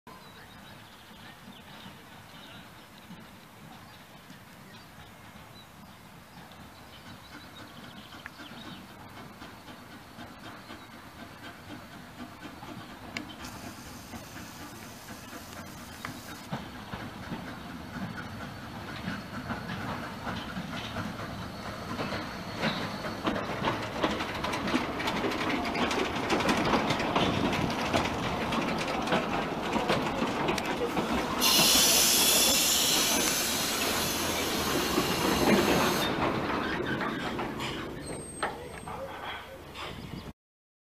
South Tynedale Railway: The No.441 ‘Barber’ on the TV in Behind The Scene was arrives at Alston.
The South Tynedale Railway at Alston: The STR’s Thomas Green & Son Ltd 0-6-2 Saddle Tank No. 441 'Barber' of 1908 with a train with the TV in Behind The Scene was arriving at the Alston on Thursday 21st April 2016